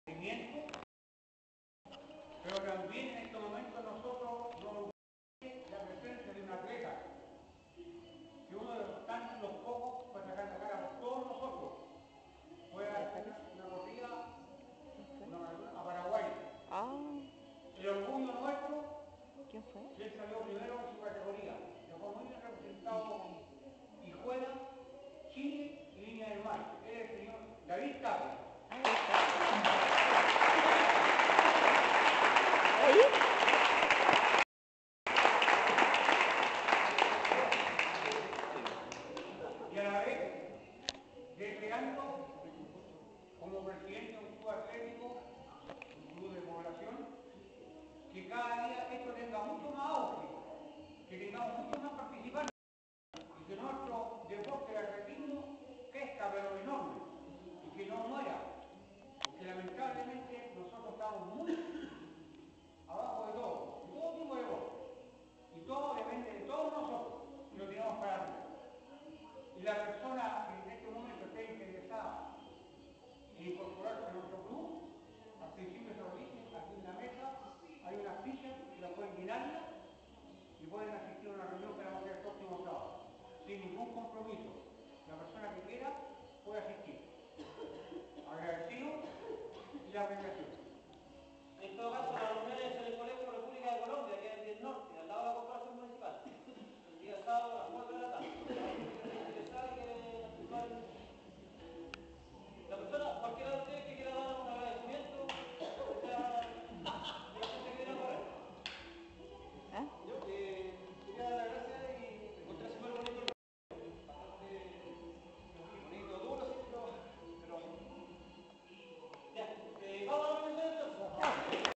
3º cross de invierno del Atletico GF Reñaca Alto
3º cross de invierno organizada por Atletico GF en Reñaca Alto Viña del Mar rodeado de hermosos paisajes corrimos 8.50 km app en el cual los GF nos deleitaron con un simpatico mago